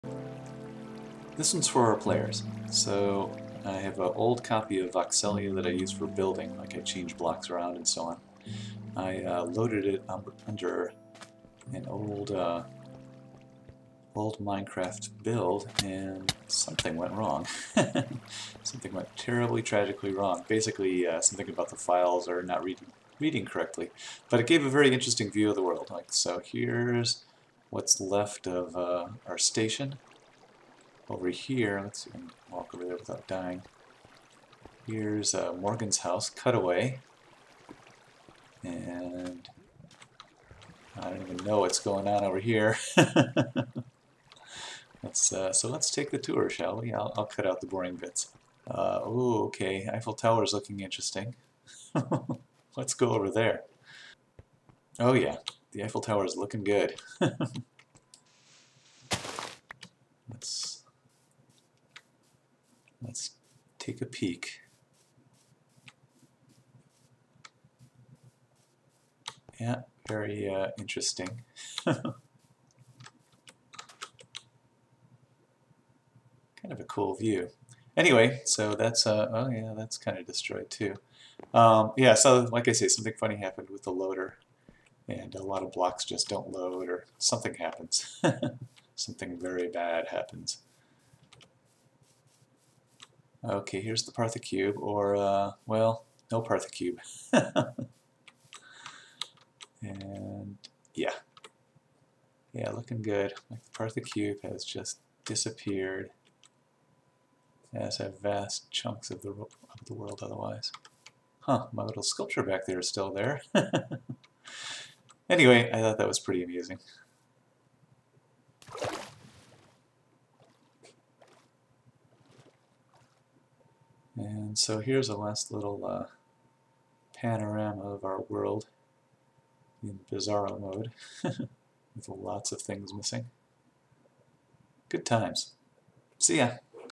Cutaway
Something odd happened today when I tried to load my local copy of Vokselia. Interestingly, each time I reload, more of the data is unloadable.